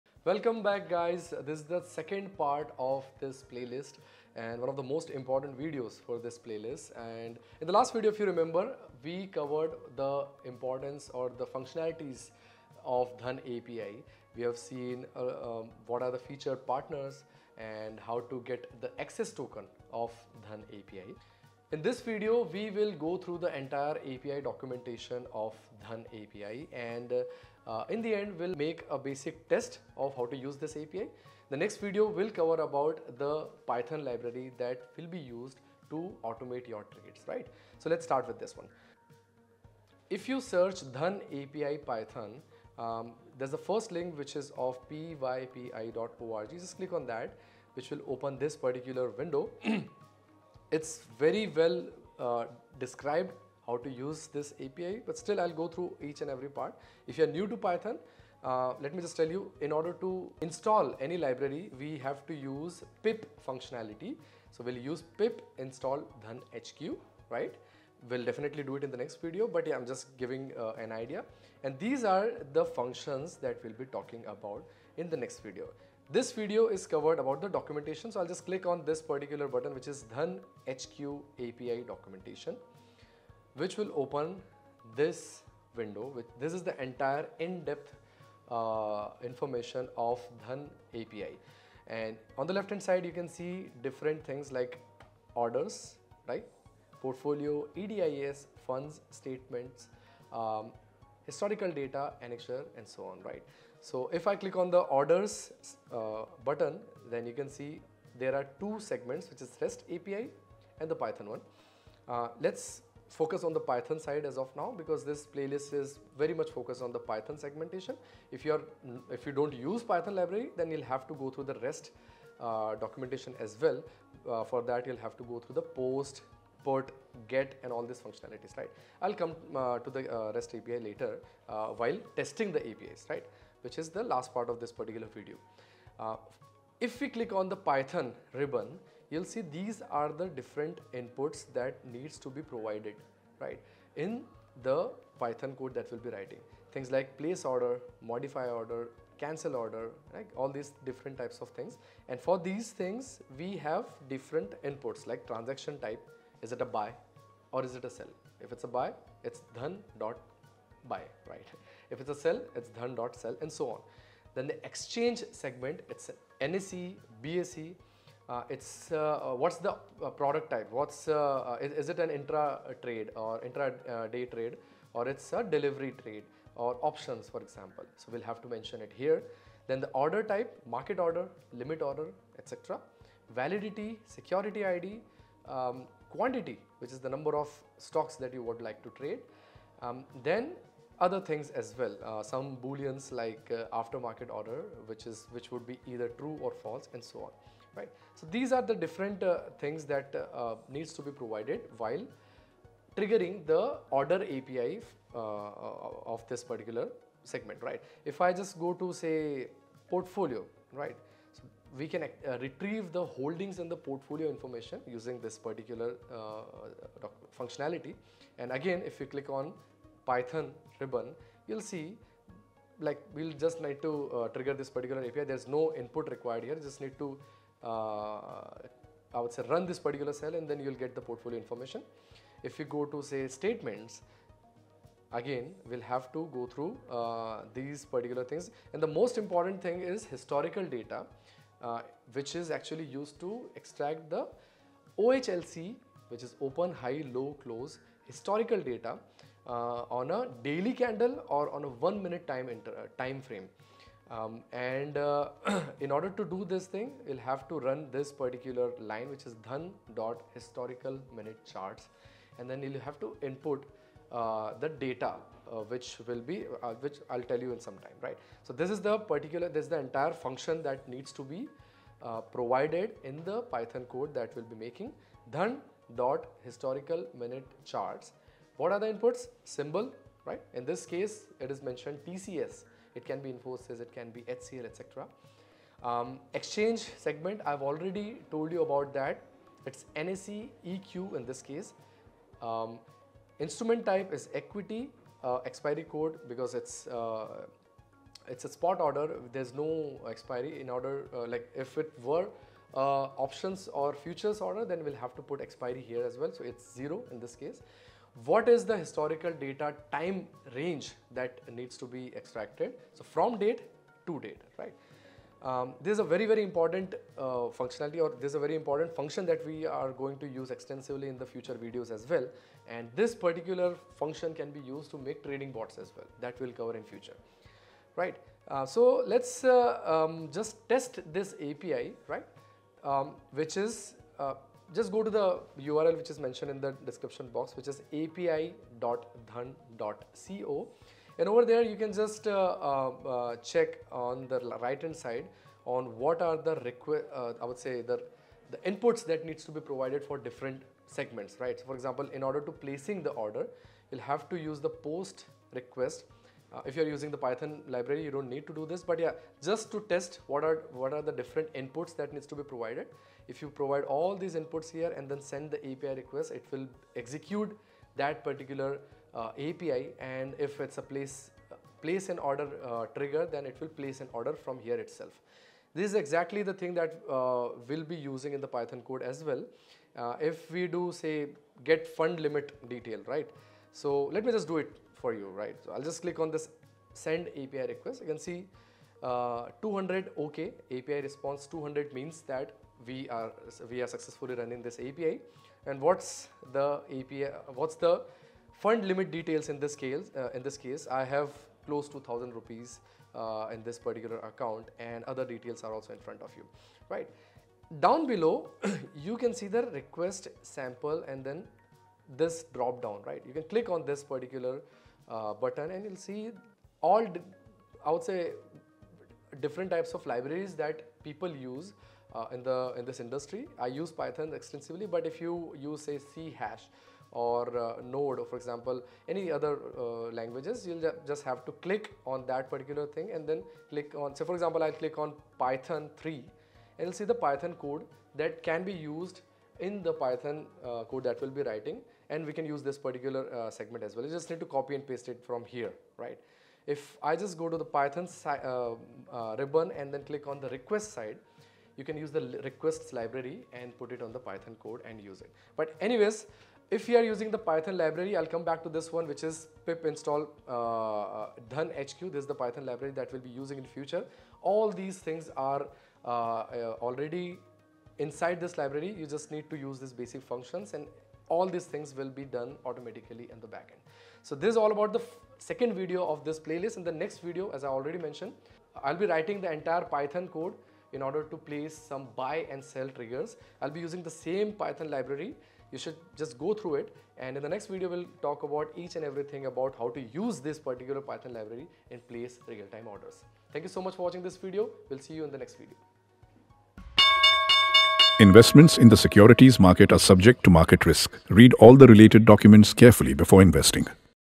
How to use an API? DhanHQ API Documentation Tutorial for Beginners in the Stock Market | Dhan
This is a tutorial video for documenting DhanHQ APIs in detail for beginners in the Stock Market.

00:40 - Python Library Introduction
01:24 - API Documentation
05:37 - Sandbox

📱Start Trading & Investing with Superfast API.